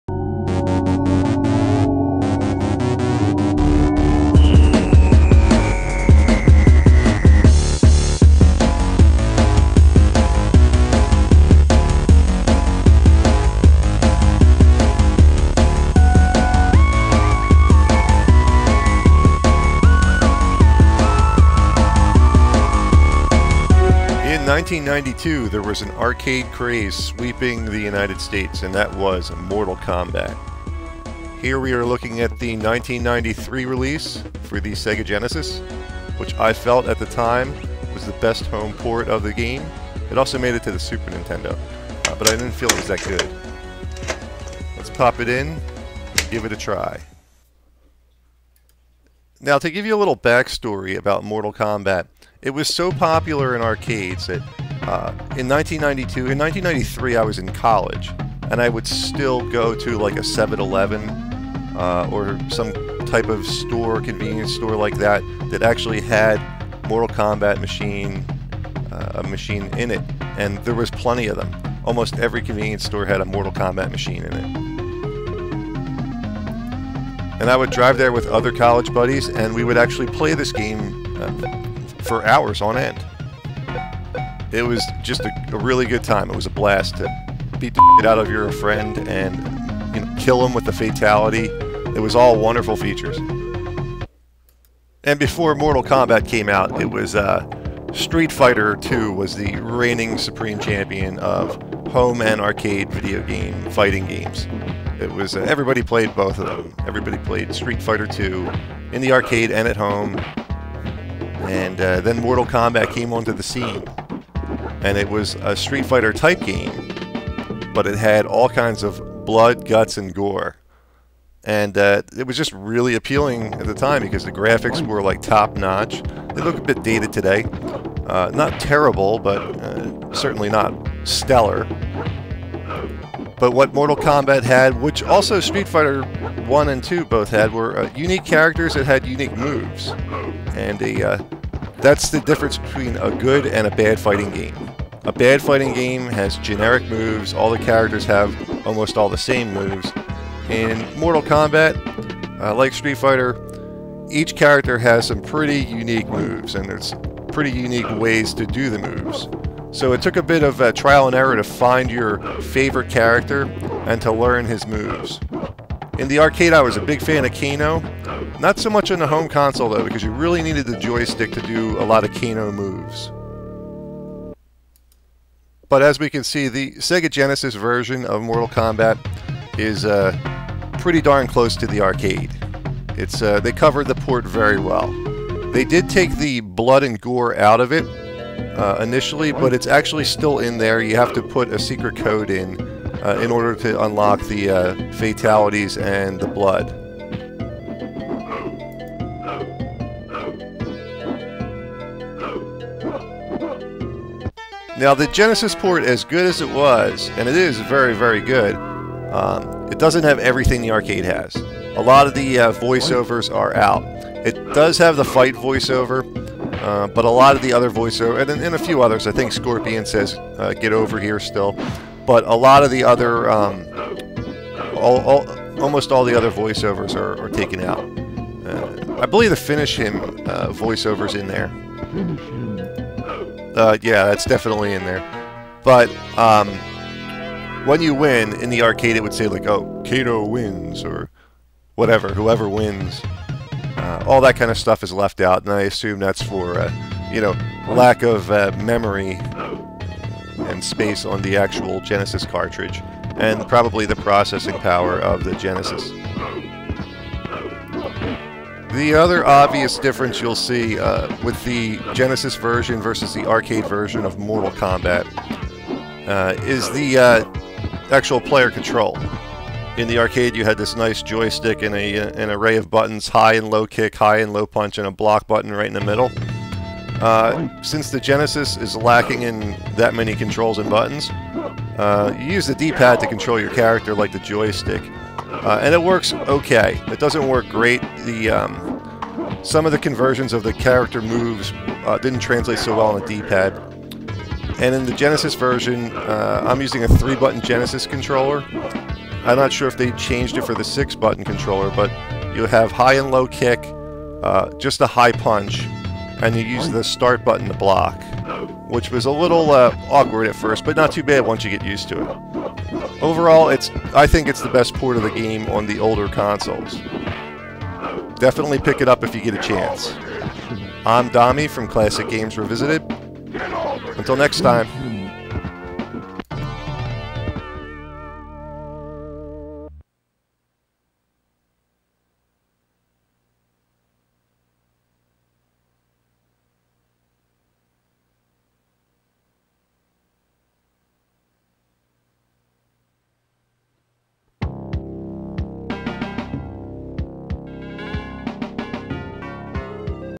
CLASSIC GAMES REVISITED - Mortal Kombat (Sega Genesis) review
This game was an instant arcade hit!  It also garnered a lot of negative media attention because of in-game violence and gore.  But how did the Sega Genesis home port of the game hold up?  Find out here!